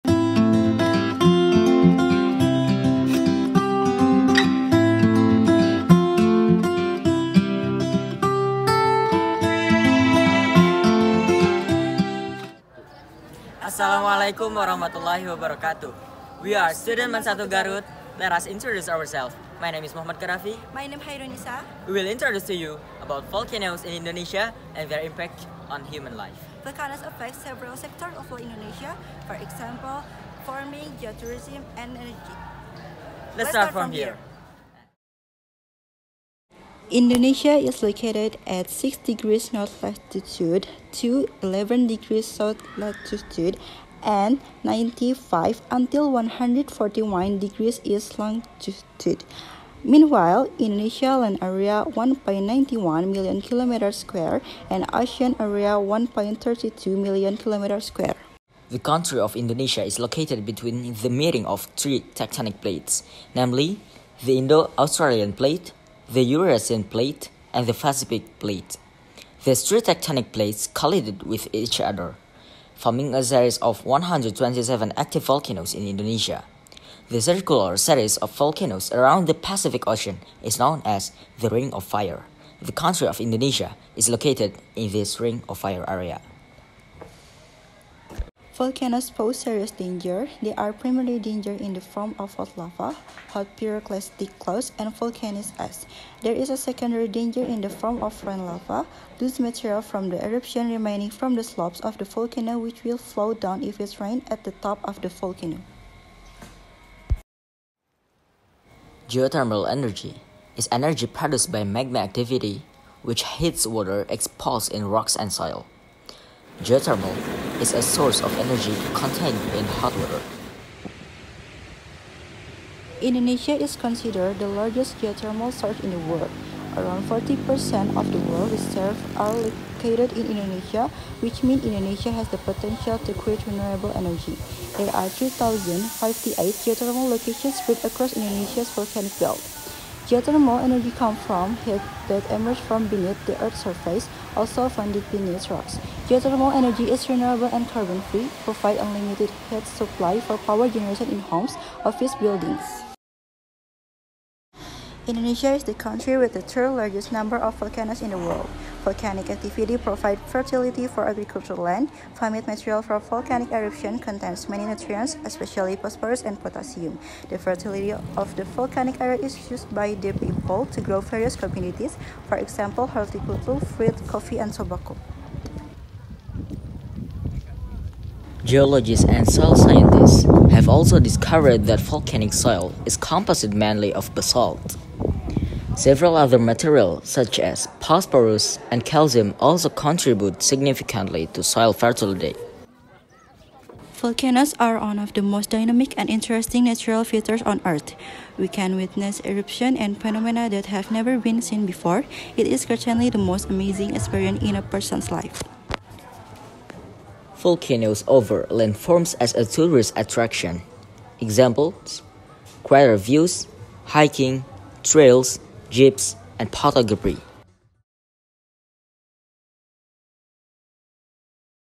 MAN 1 GARUT - M KHADAFI & KHAIRUNNISA INDONESIA : LIVING WITH VOLCANOES
Best regards student participant Geosac 2024:
1. Muhamad Khadafi
2. Khairunnisa

Video scientific based source data reference :
1. Arizona State University
2. Publikasi ilmiah Geothermal Energy Pertamina
3. BNPB Republik Indonesia
4. British Geological Survey
5. Wikipedia

Survey video location :
1. Pertamina Geothermal Energy-Kamojang, Kab. Bandung, West Java
2. Tepas Papandayan, Kec. Cisurupan, Garut
3. Barugantung, Kec. Bayongbong, Garut
4. Margawati, Kec. Garuk Kota